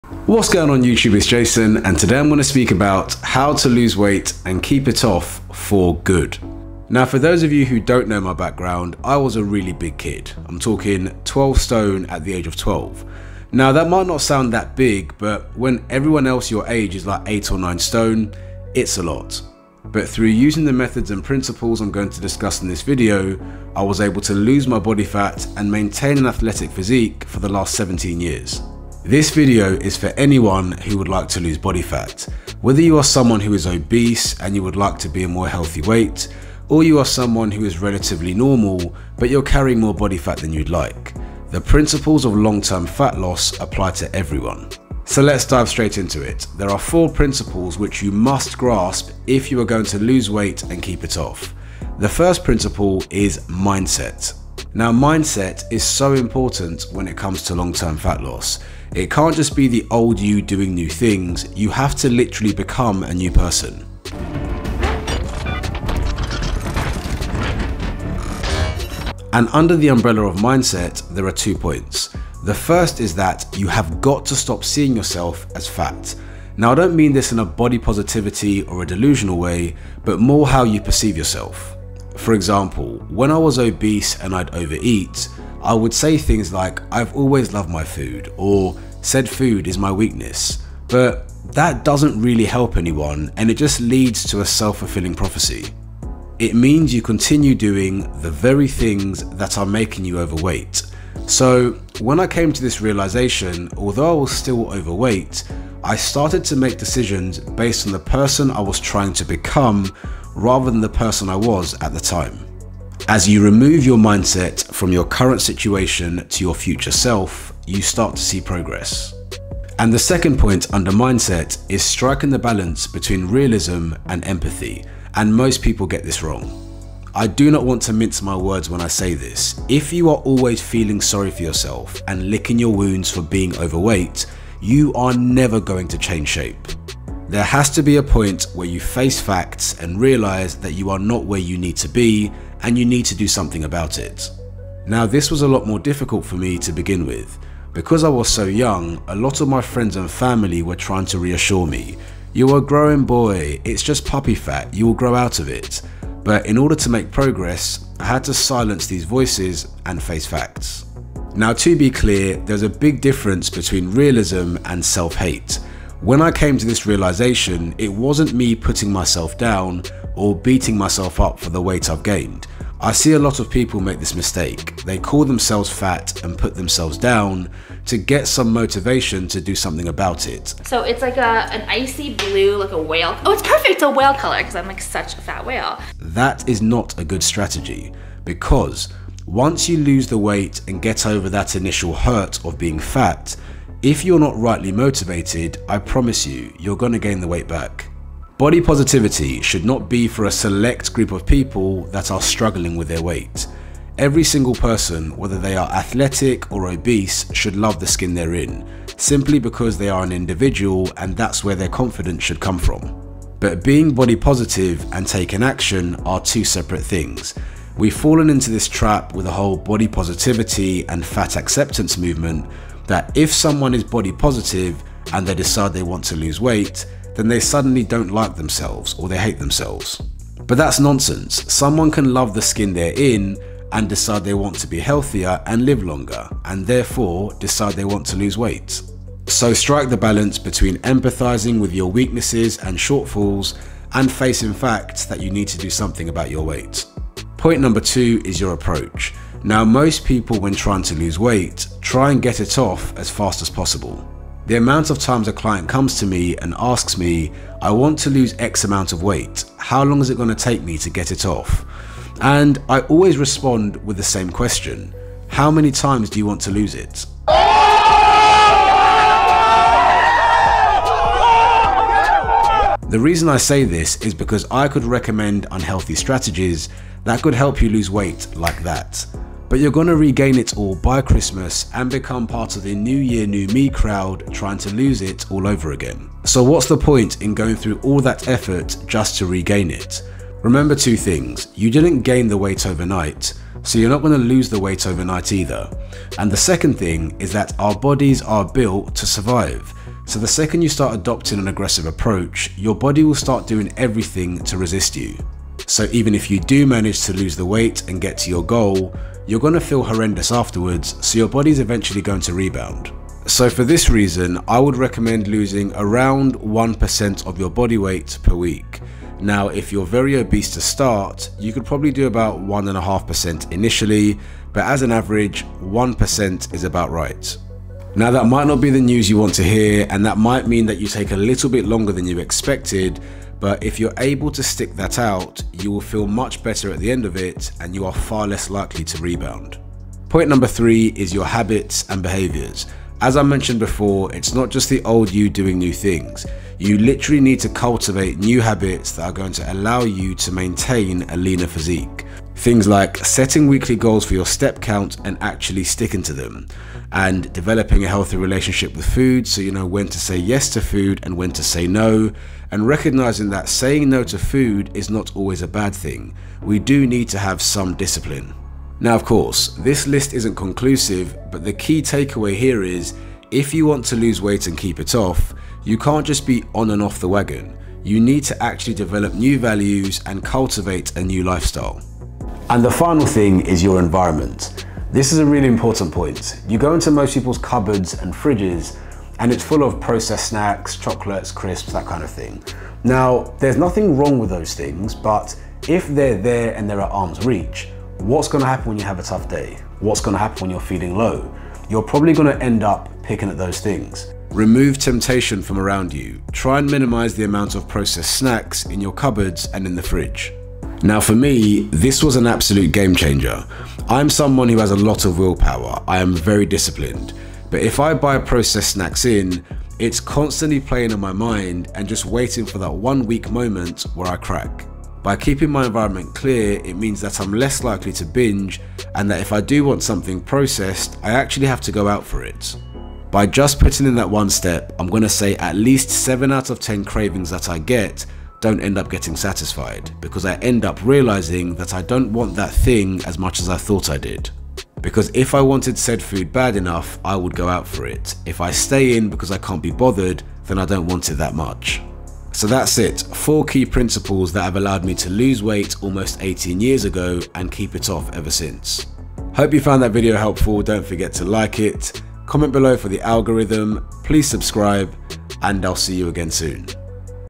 How To Lose Weight And Keep It Off - FOREVER!!!
In this video I go through 4 key principles that have allowed me to lose over 20kg and maintain an athletic physique for over 17 years!